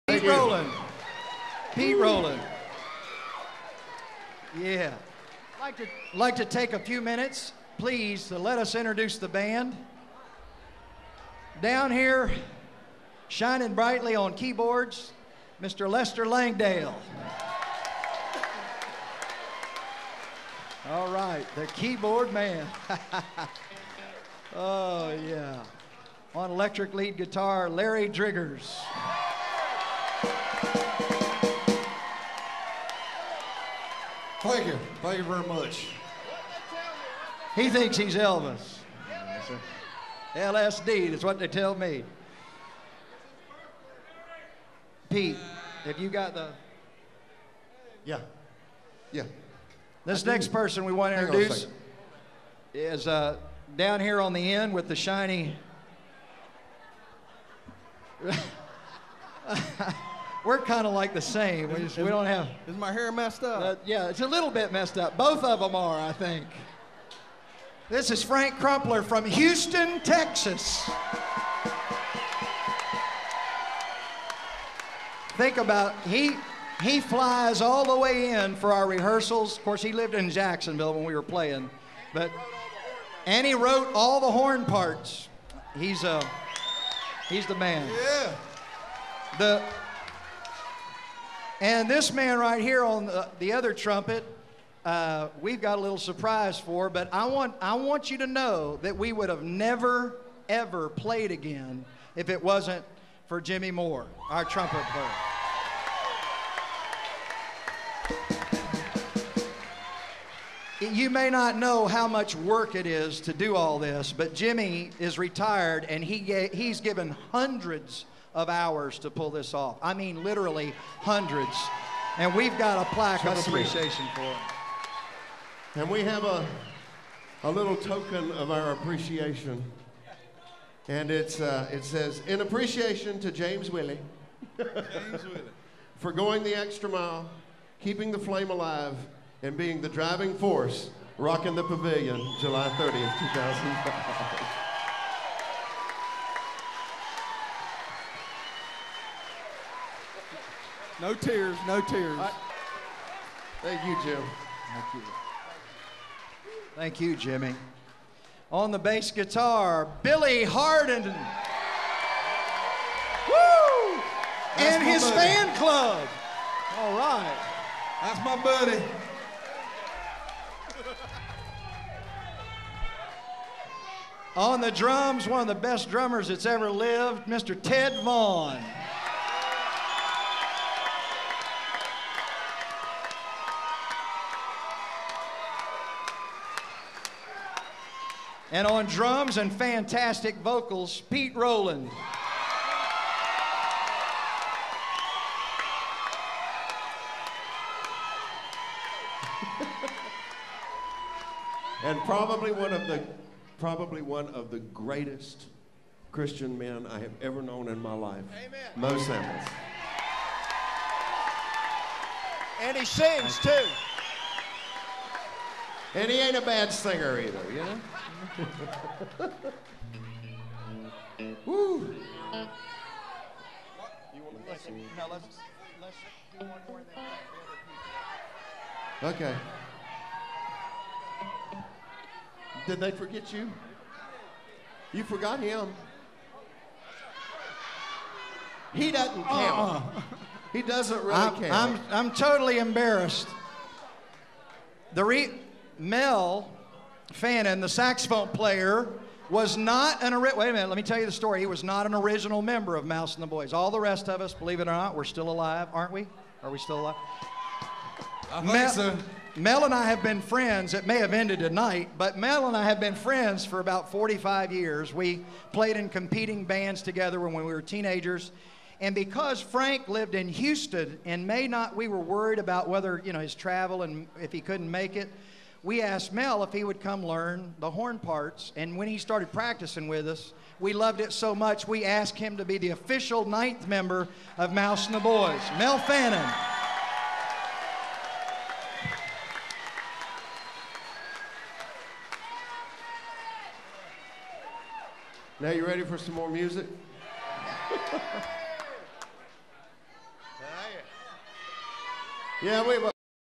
Mouse and the Boys Band Member Introductions
Mouse takes a couple of minutes to introduce the band members to the crowd.  Jax FL 2005 Second Set.